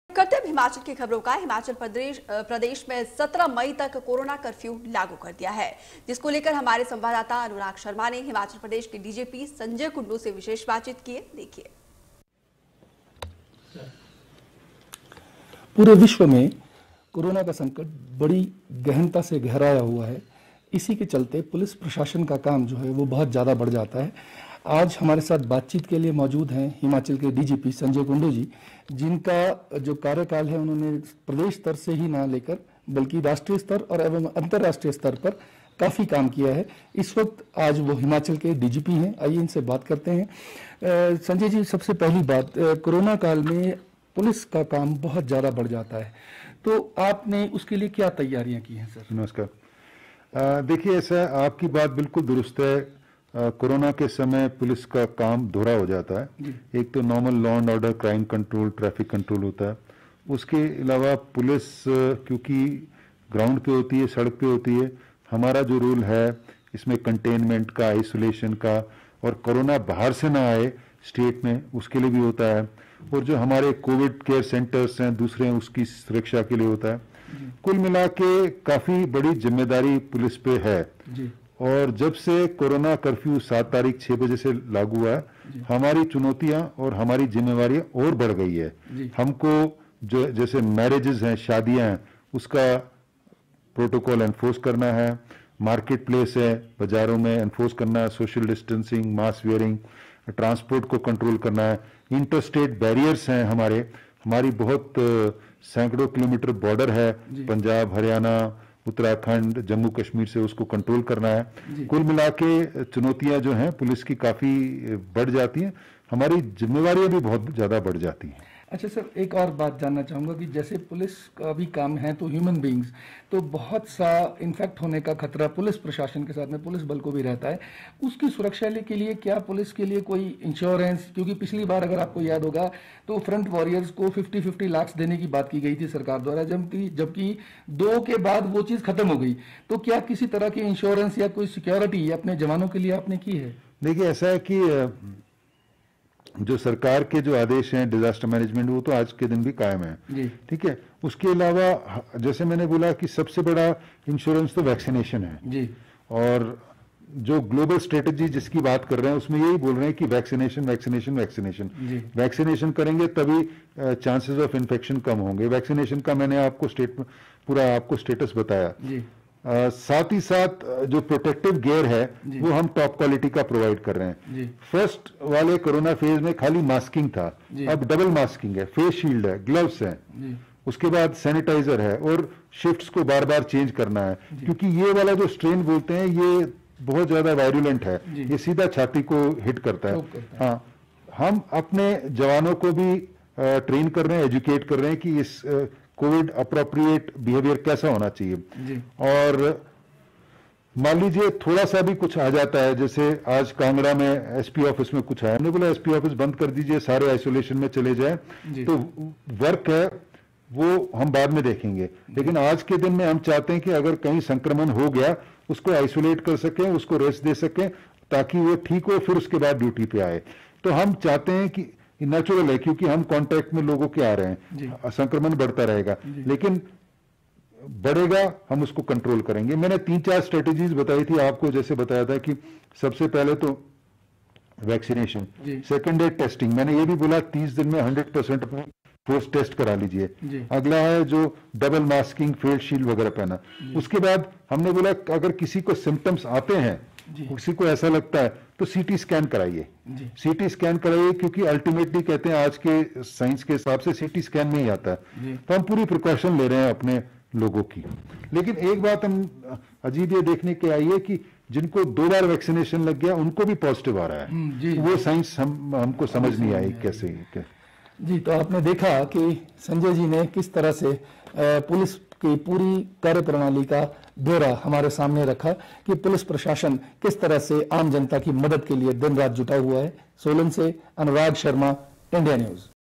Himachal : सोलन - लोगो को कोरोना के प्रति कर रहे जागरूक - संजय कुंडू
Himachal : सोलन - लोगो को कोरोना के प्रति कर रहे जागरूक - संजय कुंडू |India News  Himachal